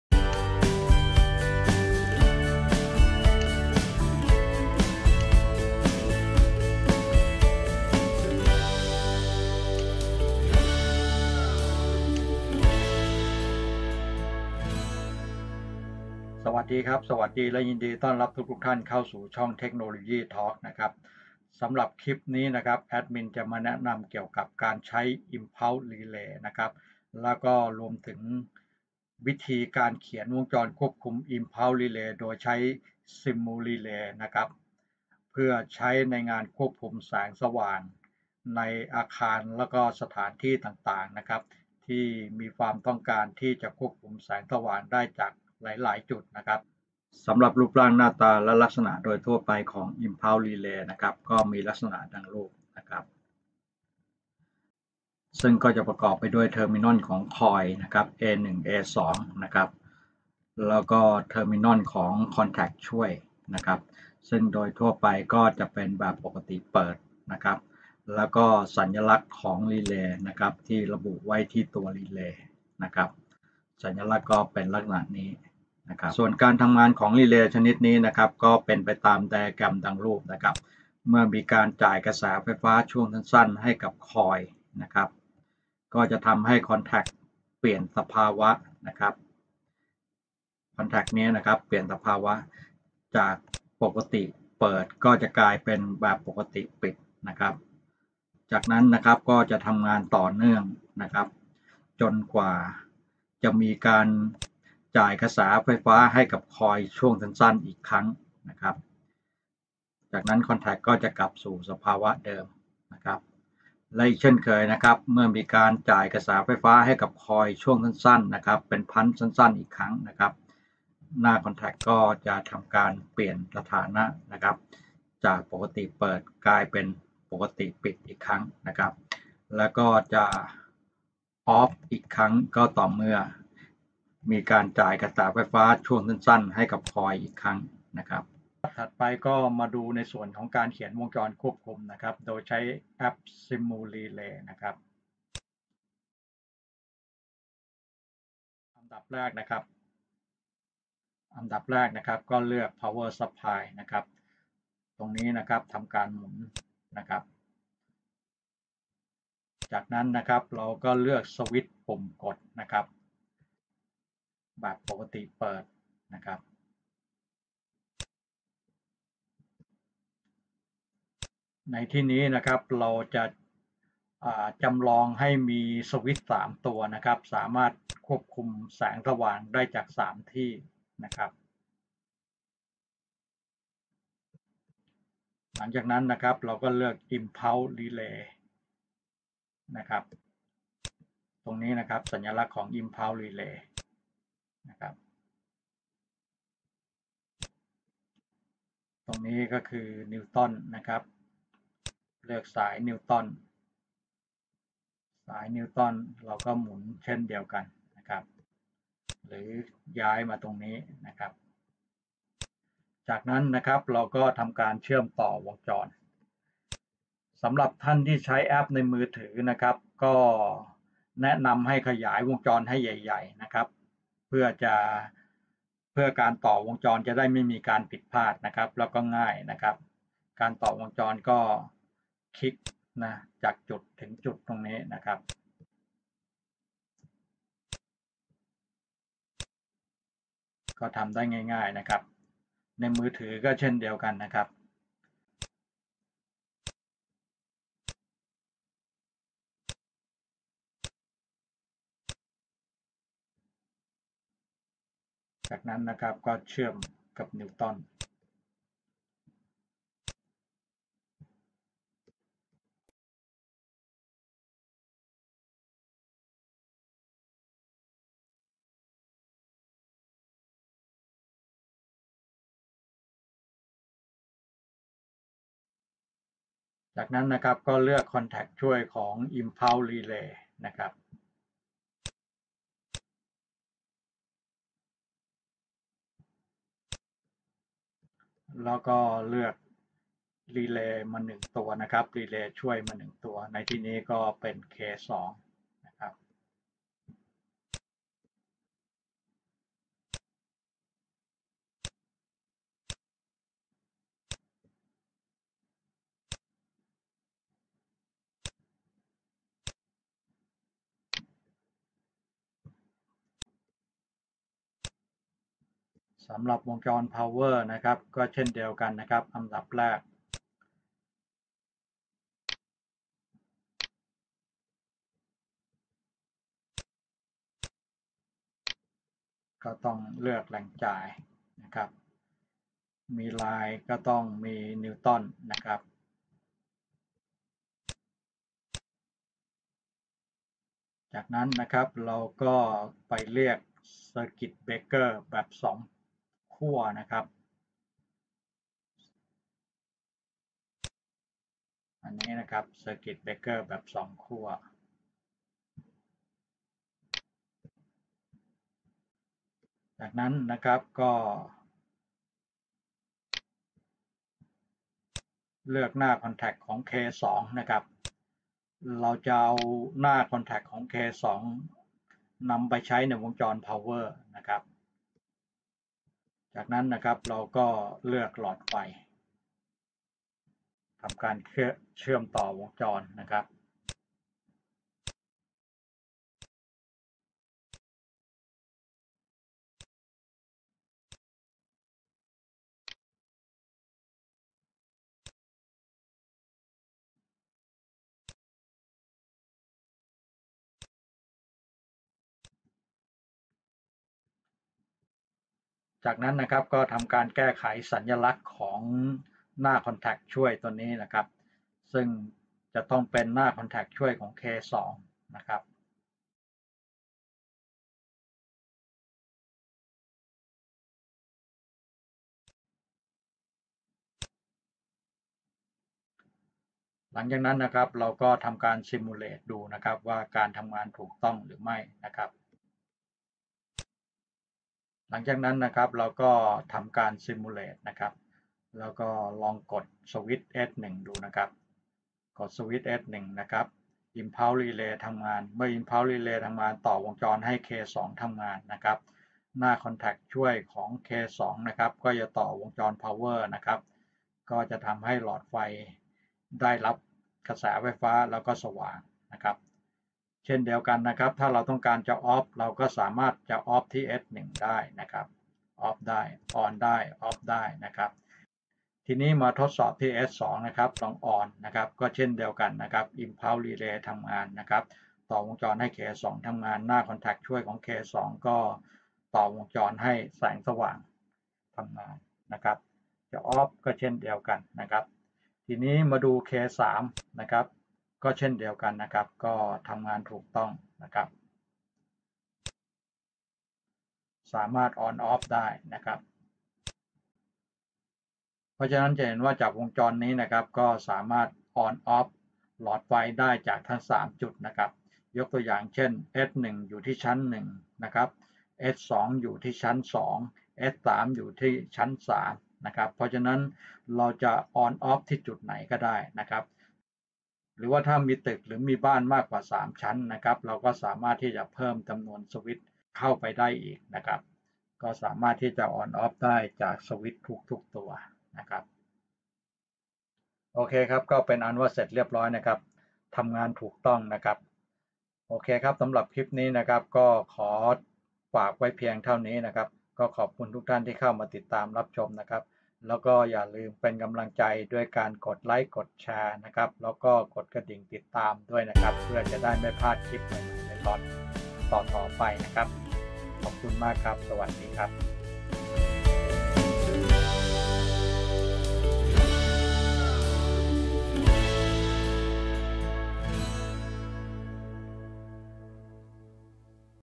multi point Lighting Control by using Impulse Relay การควบแสงสว่างแบบหลายๆจุดโดยใช้อิมพัลส์รีเลย์
แนะนำการควบระบบแสงสว่างแบบหลายๆจุดโดยใช้อิมพัลส์รีเลย์ และสาธิตการเขียนวงจรควบคุมด้วยแอพ Simurelay

ขอบคุณทุกๆท่านที่เข้ามาติดตามรับชม  รบกวนช่วยกดซับสไคร ( Subscribe)  กดไลค์ กดแชร์ และกดกระดิ่งติดตามเพื่อเป็นกำลังใจให้แอดมินหน่อยนะครับ